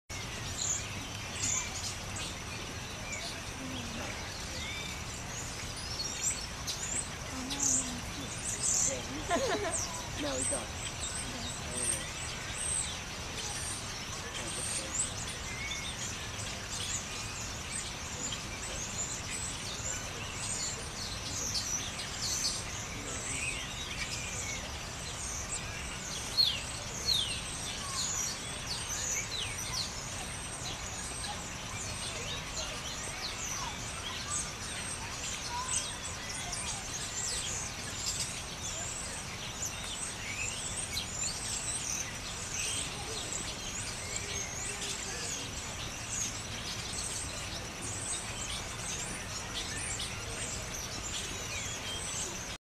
Philadelphia Zoo Black Necked Swan in Beautiful Scene
This black necked swan is one of my special favorites in the Zoo.  And I love beautiful, peaceful scenes like this.